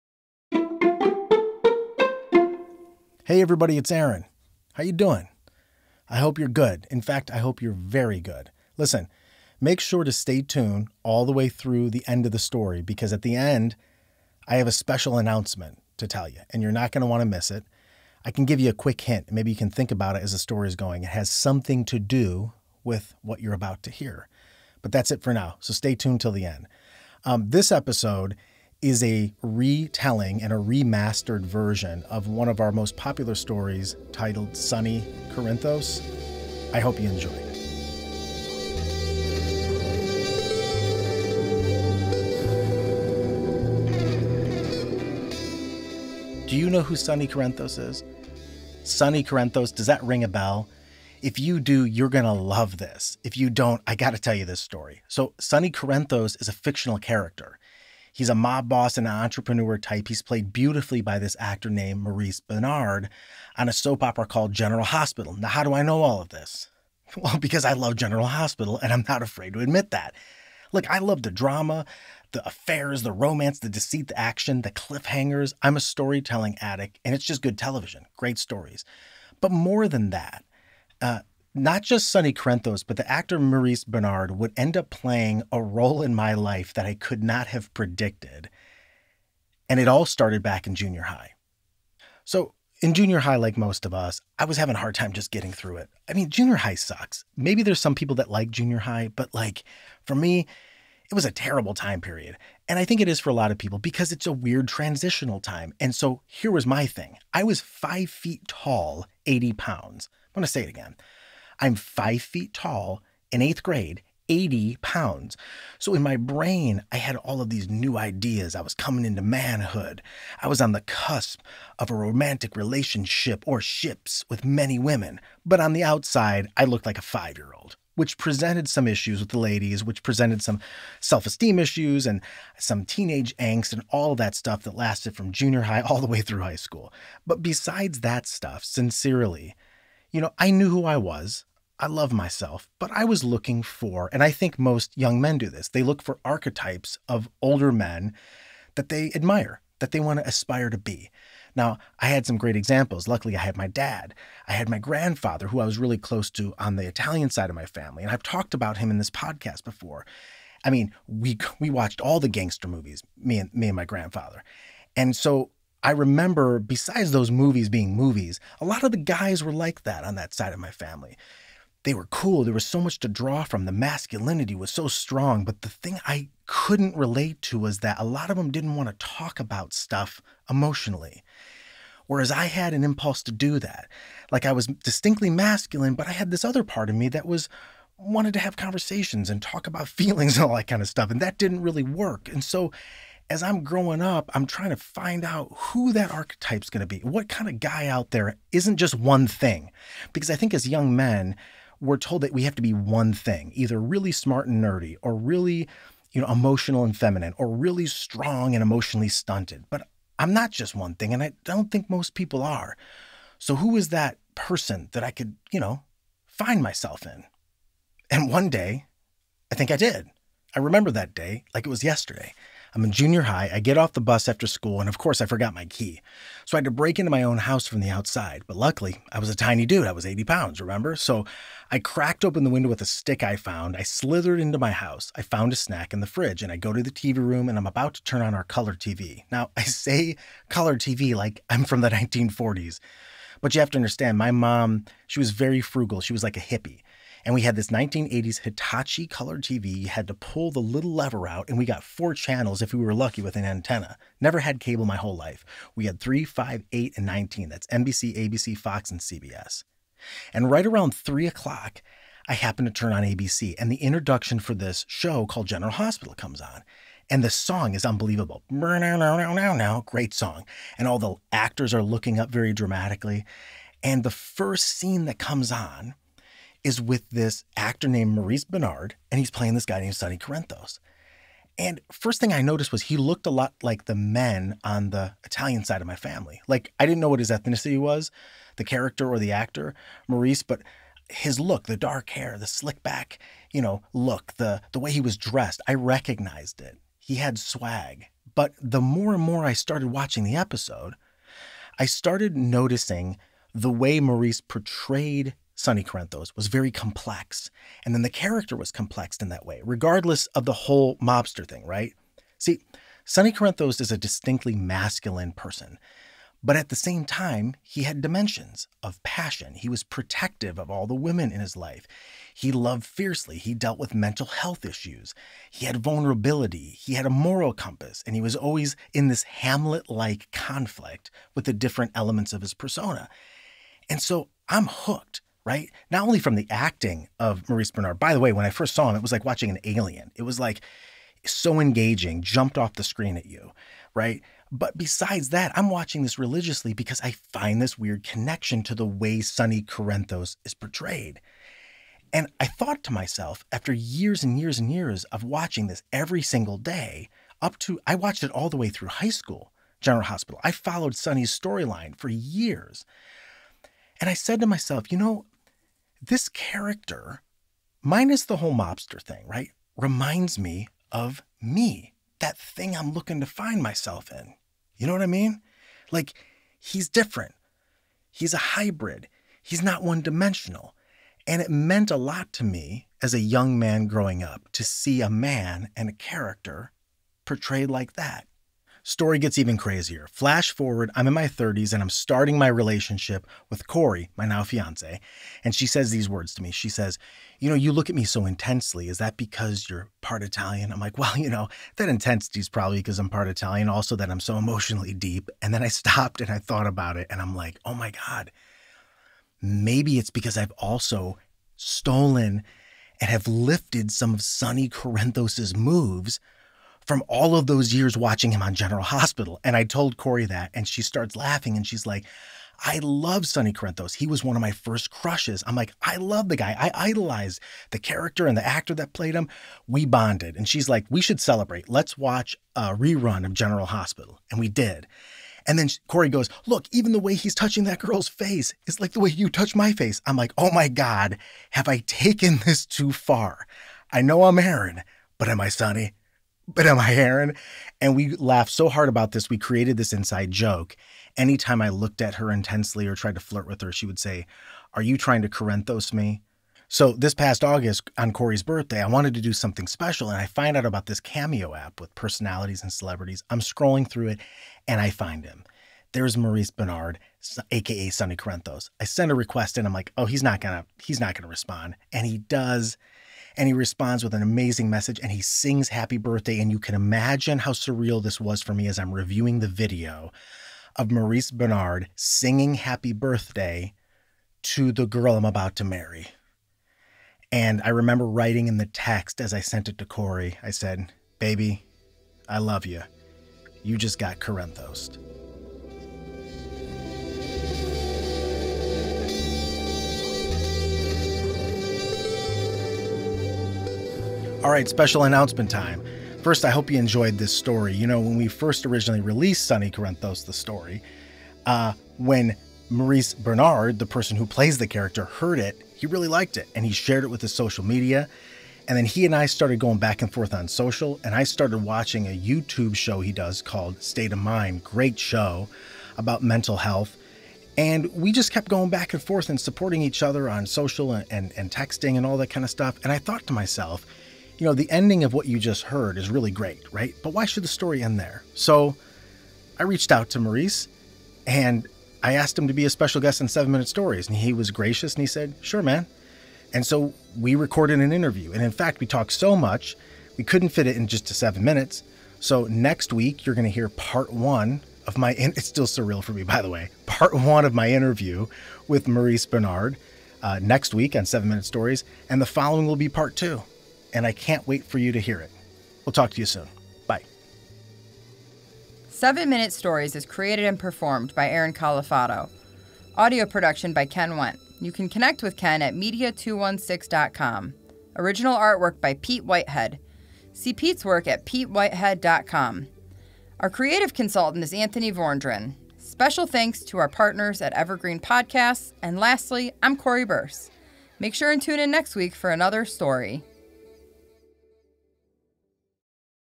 Episode 127 - Sonny Corinthos OR Sonny with a chance of Aaron
The story of how a one of a kind soap opera character made an unexpected impact on my life.

Story concept & performance by: Aaron Calafato

Audio Production: Ken Wendt

Original Art: Pete Whitehead

Music Contributor: thomas j. duke

Podcast Coordinator: Cori Birce

Creative Consultant: Anthony Vorndran